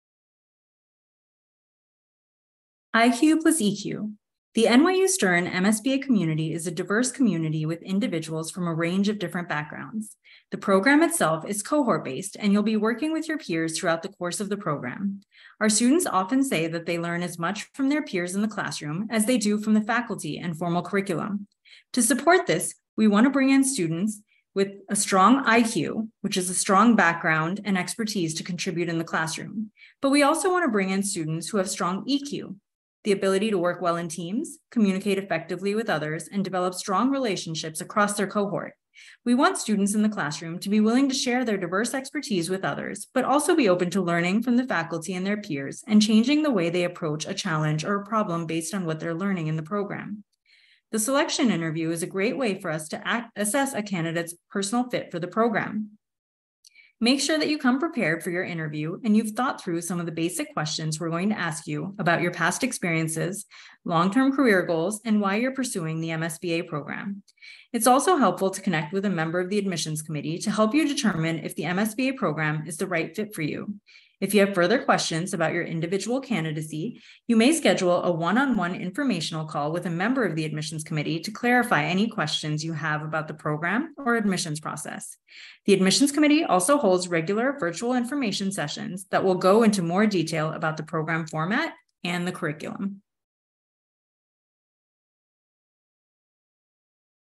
NYU Stern MSBAi Application Tips: Focusing on IQ + EQ
The NYU Stern MS in Business Analytics and AI program is a one-year, part-time program designed for full-time working professionals. To learn more about the Admissions Criteria for the MSBAi program, please visit: stern.nyu.edu/MSBA-Apply